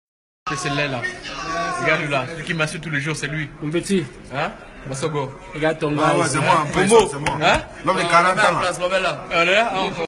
Quand Christian Bassogog ironise sur son âge et s’attribue 40 ans
Quand Christian Bassogog ironise sur son âge et s’attribue 40 ans  (Vidéo)